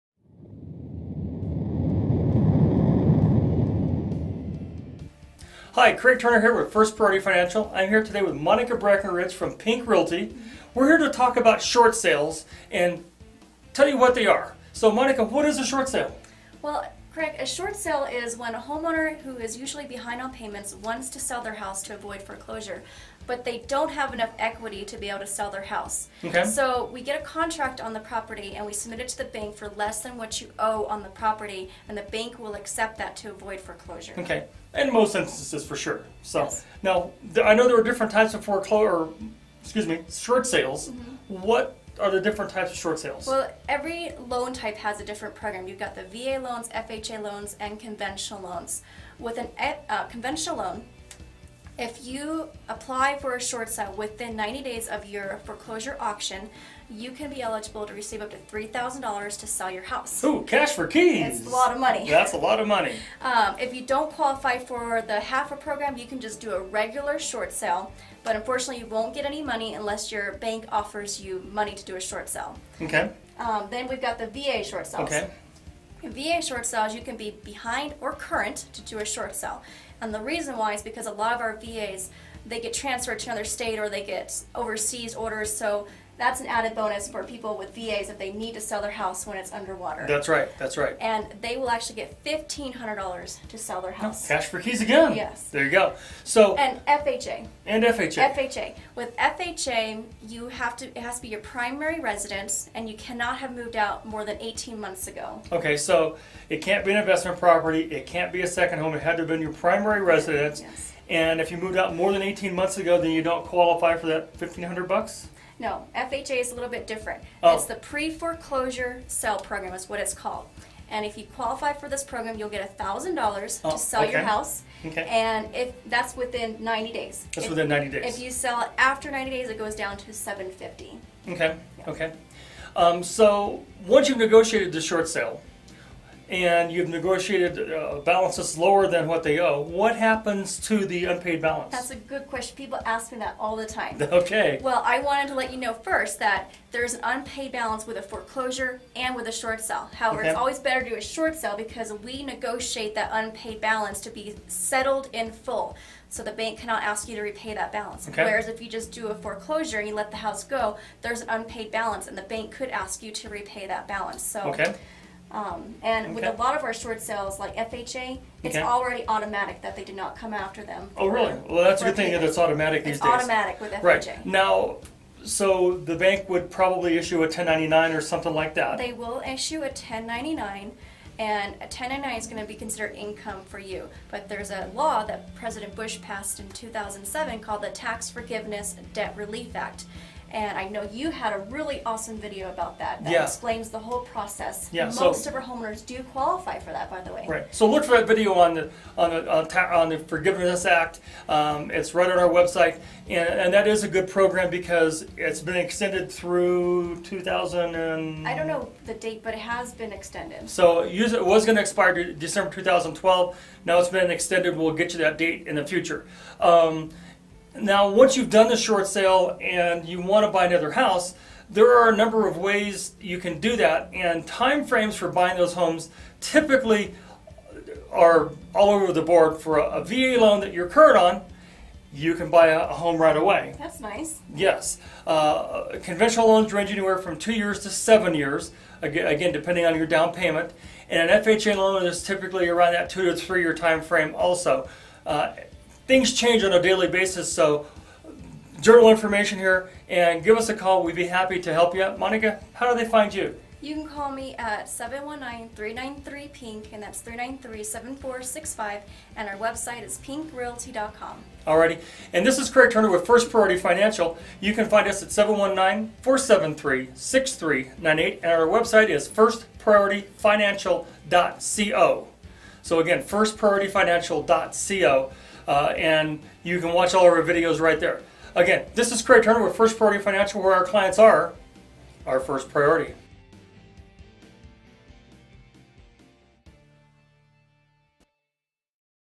What You Need to Know About Short Sales and Buying a New Home!
This is an interview Monica Breckenridge owner of Pink Realty and Short Sale Specialist had with Craig Turner at First Priority Financial.  I answer the following Questions:

1.  What is a short sale
2.  What are the different types of short sales
3.  What happens to the unpaid balance
4.  Do you get a 1099

Craig goes over getting a new loan after a short sale.

You can reach Craig Turner at
First Priority Financial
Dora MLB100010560
NMLS # 187319